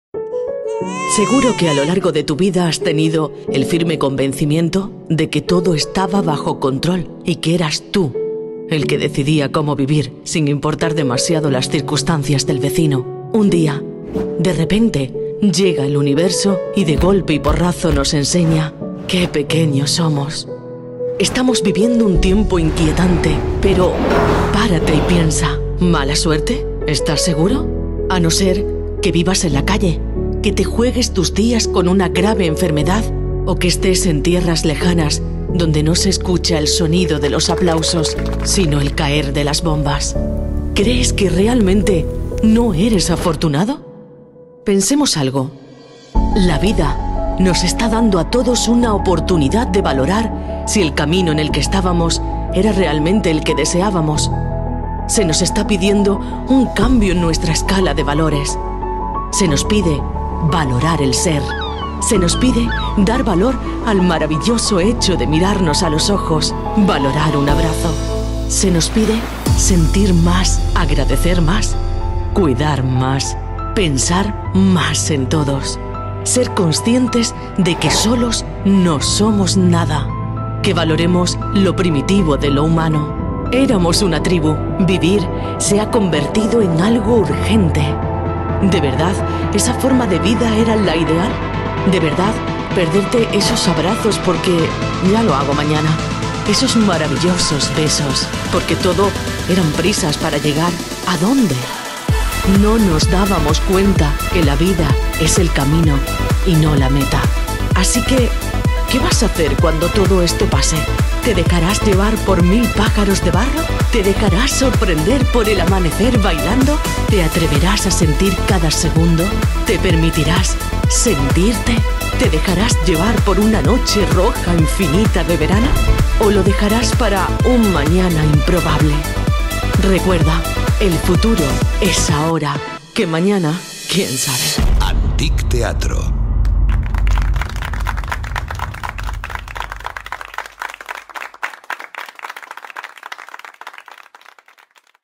EL FUTURO ES AHORA
Desde la dirección de Antique Theatro, hemos querido unirnos en esta campaña contra el coronavirus, creando un vídeo de concienciación con todo nuestro cariño. Esperamos que os guste y que os ayude a sobrellevar estos días de confinamiento. Para vosotros y con todo el amor de nuestro equipo 👇🏻⁣⁣
De repente la vida nos obliga a parar. En estos días aprovechamos para reflexionar✨⁣⁣
Ahora es el momento de pensar ¿estamos en el camino correcto? Nuestras prioridades del día a día han cambiado, y lo que antes dejábamos para mañana hoy es lo más importante.⁣⁣
Un abrazo, una sonrisa, una mirada; las pequeñas cosas se convierten en grandes para enseñarnos de qué está hecha la esencia de la vida🖤⁣⁣
Que estos días nos sirvan para recordar la razón por la que estamos aquí. La vida nos está dando una lección y en Antique Theatro la hemos aprendido: el futuro es ahora⏱⁣⁣
Ríe, sueña, disfruta; la vida y nosotros pronto os esperamos, pero mientras tanto